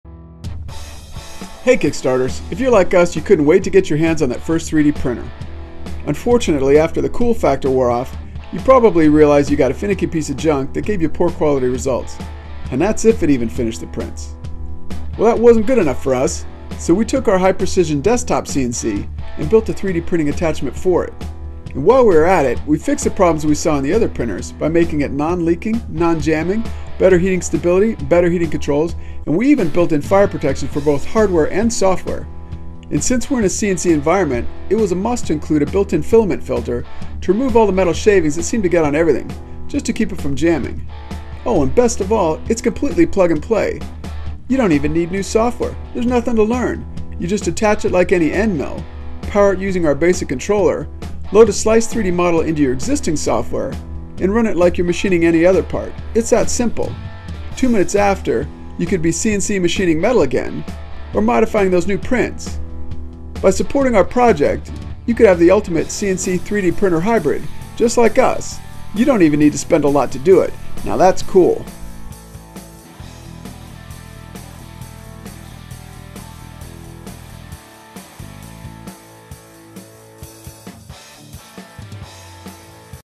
Plug-n-Play 3D printing attachment for CNC Mills
Imagine swapping in/out a 3D printing attachment like you would any end mill for your CNC ... You've just created the ultimate Hybrid!

AVAILABLE ON KICKSTARTER NOW!